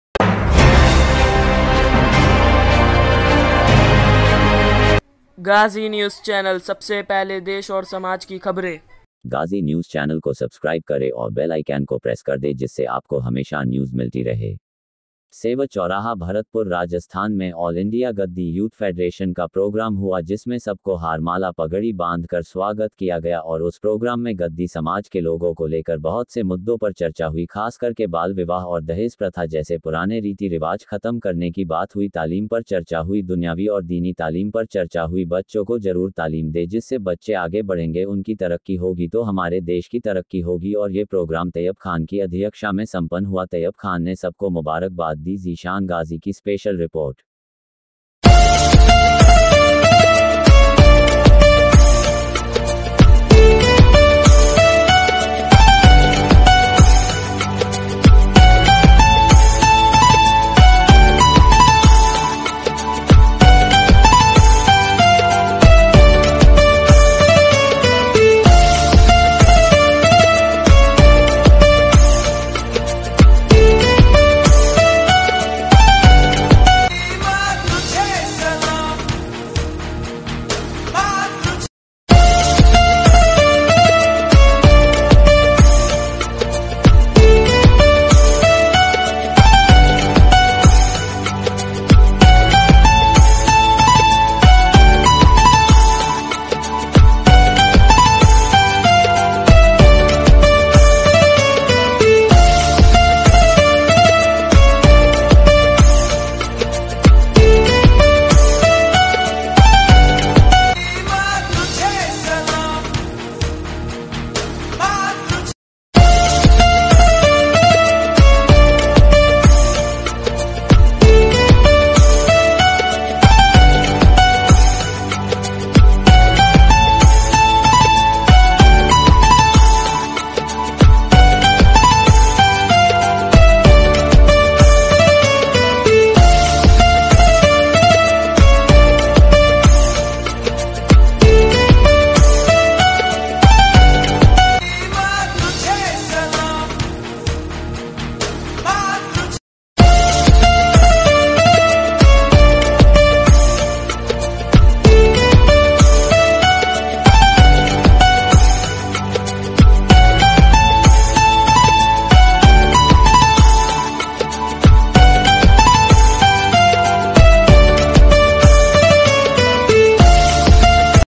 GhaziNC सेवर चौराहा भरतपुर राजस्थान में AIGYF की टीम का ज़ोरदार स्वागत हुआ.तैयब खान की अध्यक्षा....
सेवर चौराहा भरतपुर राजस्थान में ऑल इंडिया गद्दी यूथ फेडरेशन की टीम का ज़ोरदार स्वागत कार्यक्रम हुआ.तैयब खान की अध्यक्षा में संपन्न हुआ

give big thumbs 👍...

tell me in comment box how was the video...

ALL INDIA GADDI SAMAJ FEDERATION.

ALL INDAI YOUTH FEDERATION.